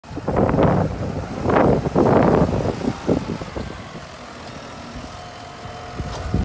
Sneeuwruimen Purmerring door Cor Beets Group
Op zondag 7 februari lag er zo veel sneeuw dat we de hulp hebben ingeroepen van Cor Beets.